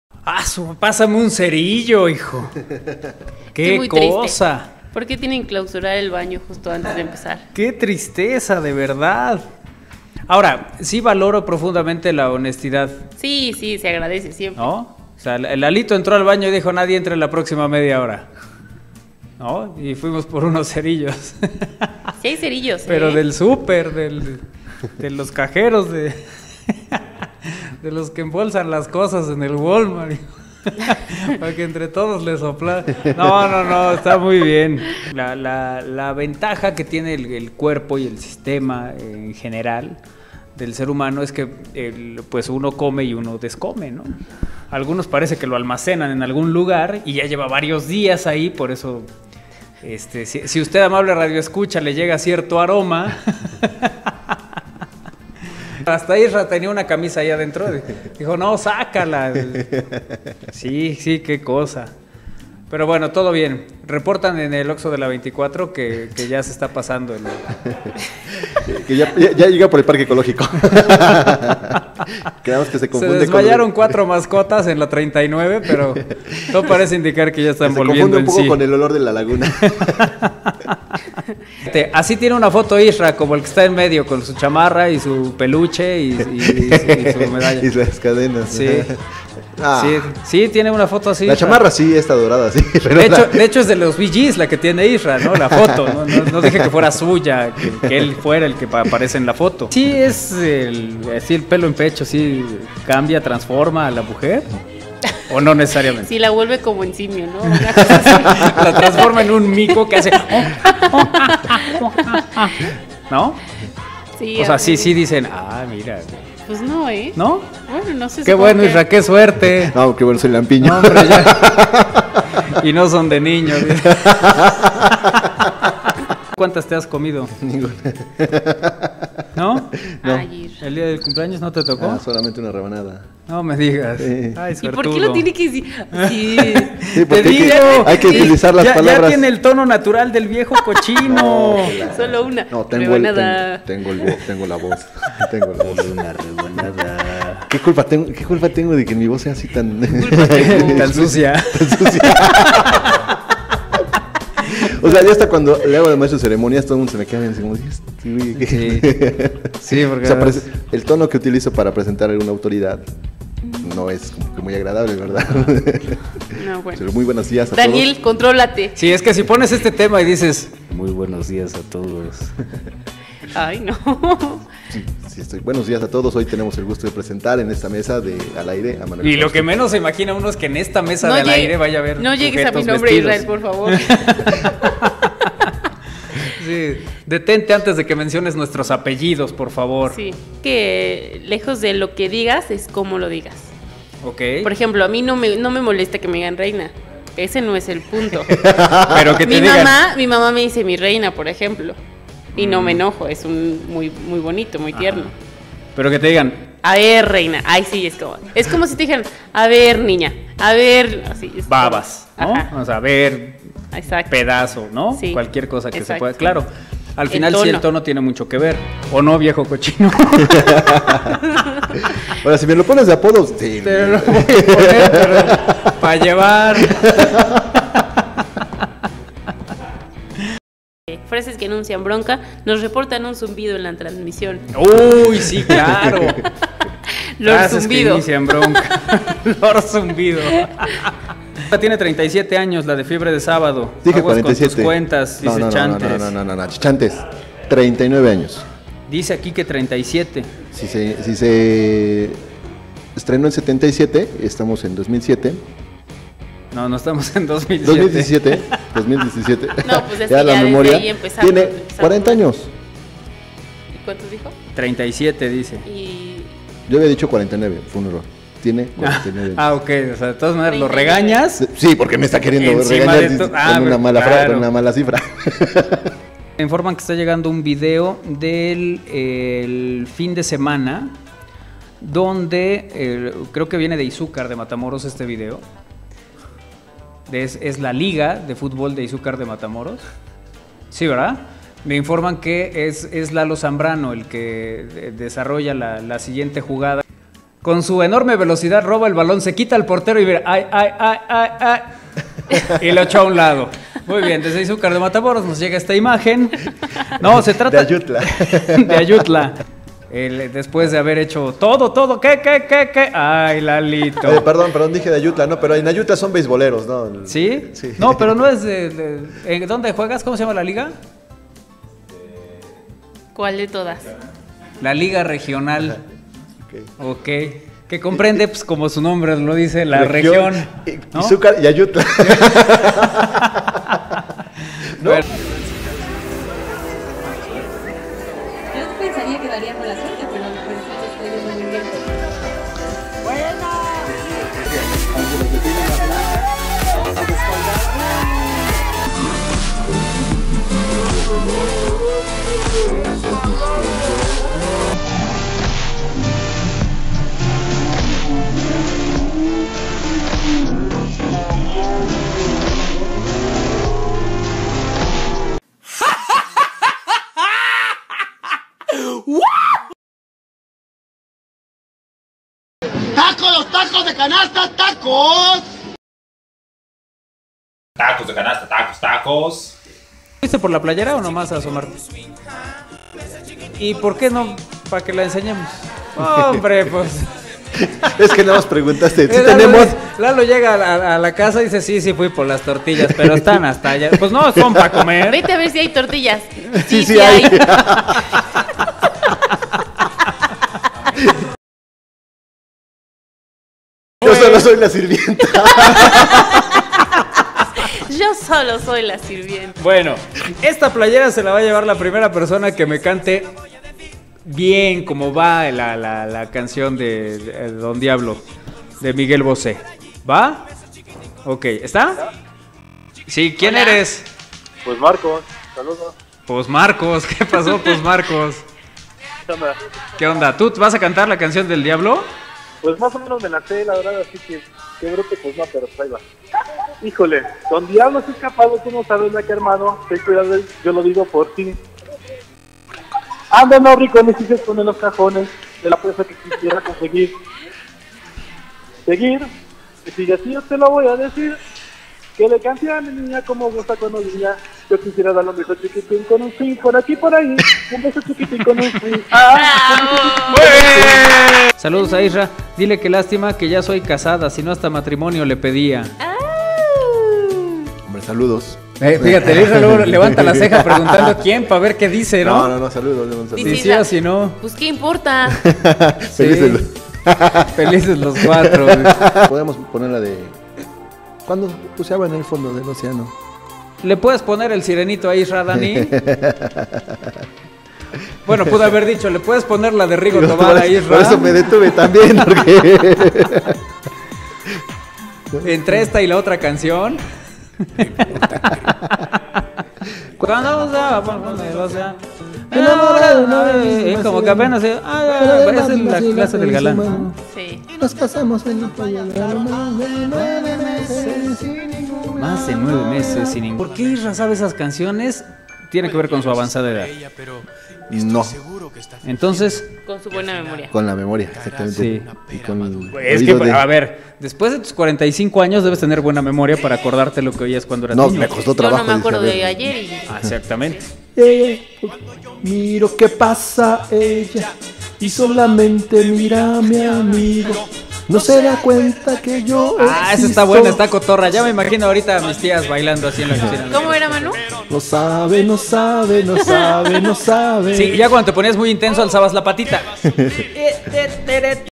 Isra cantante de Bee Gees
No te puedes perder los bloopers de la semana. Isra cantaba con el grupo Bee Gees, y tenemos la razón de porque nuestro colaborador Lalo Zambrano no va a los partidos de entrenamiento